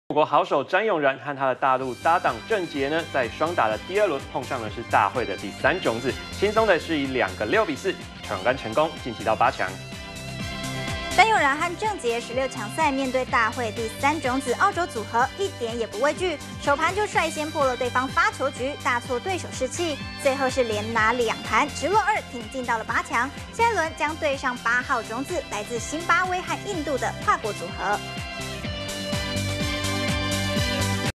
【2013.10.02】北京女網賽 詹詠然雙打闖八強 -udn tv
我國好手詹詠然和大陸搭擋鄭潔，雙打第二輪，碰上大會第三種子，輕鬆地以兩個6比4，闖關成功，晉級八強。詹詠然和鄭潔，16強賽面對大會第三種子澳洲組合，一點也不畏懼，首盤率先破了對方發球局，大挫對手士氣，最後連拿兩盤，直落二挺進八強，下一輪將對上八號種子，來自辛巴威和印度的跨國組合。